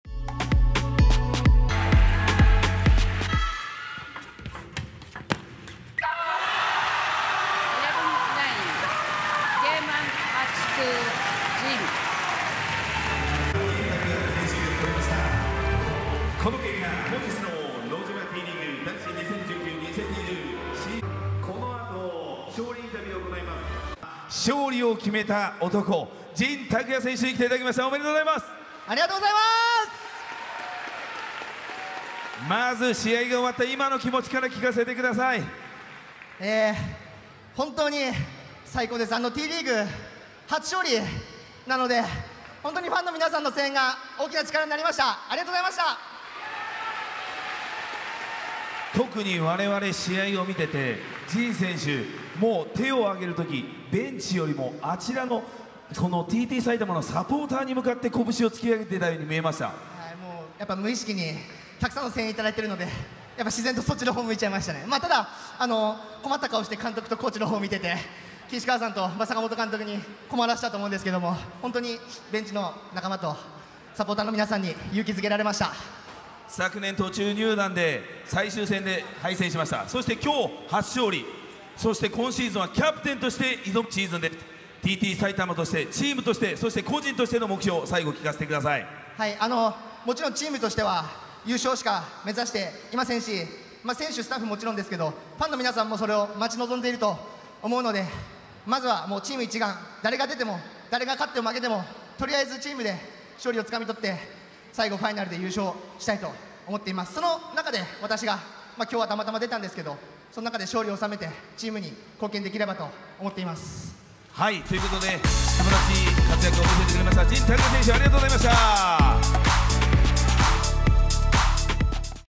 【卓球 Tリーグ公式】Tラリー 神 巧也 vs チュ セヒョク Takuya Jin vs Joo Sae-Hyuk 2019 T.LEAGUE
Tラリーは、「とっても白熱したTリーグのラリー」の略！
男子開幕2日目は、彩たまとアスティーダの初戦！2年目のシーズンはキャプテンとして臨む彩たまジンタク！そしてアスティーダはレジェンドチュセヒョク！初戦から気迫のみなぎる試合に！神巧也選手 vs チュセヒョク選手のラリーを！お楽しみください！

Takuya Jin vs Joo Sae-Hyuk  2019 T.LEAGUE
2019年8月30日（金）ノジマＴリーグ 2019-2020シーズン男子  T.T彩たま VS 琉球アスティーダ